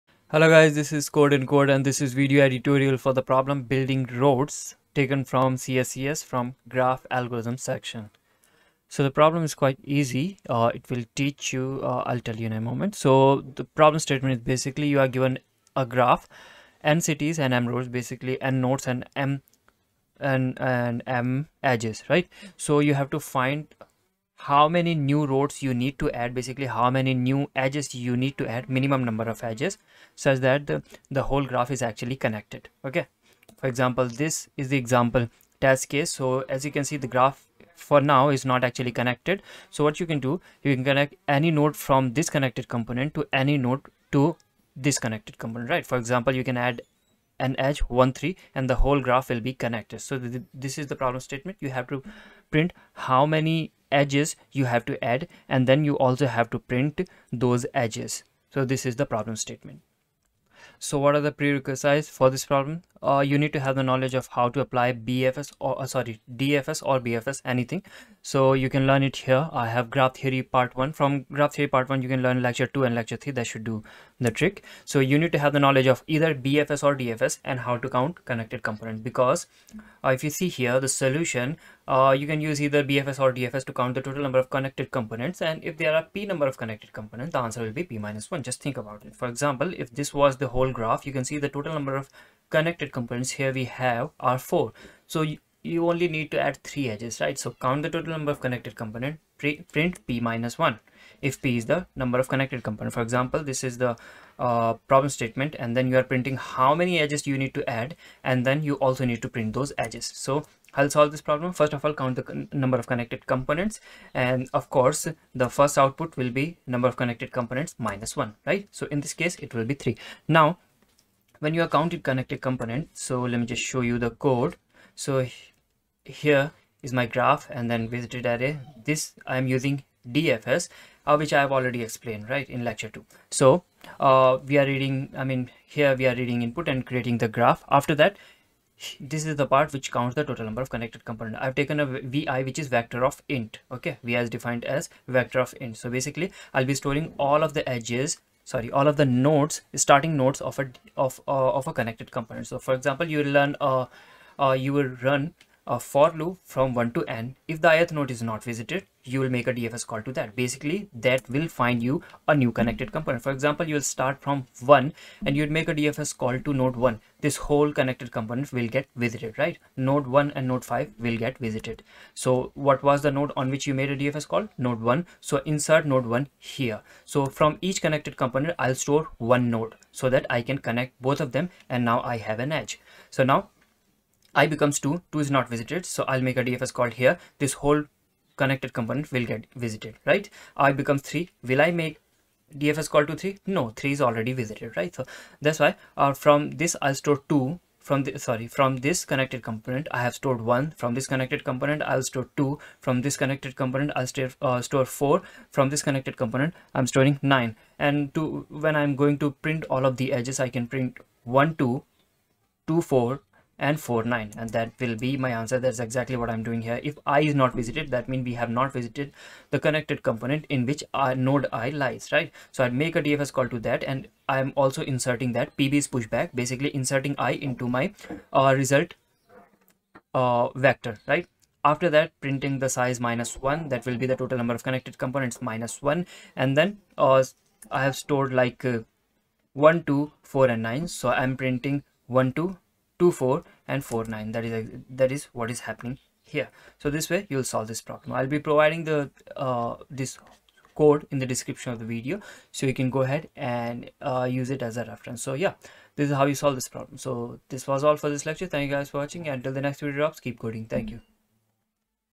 E02 : Building Roads | Graph Algorithms | CodeNCode
In this lecture we will solve problem "Building Roads" taken from CSES graph algorithms section.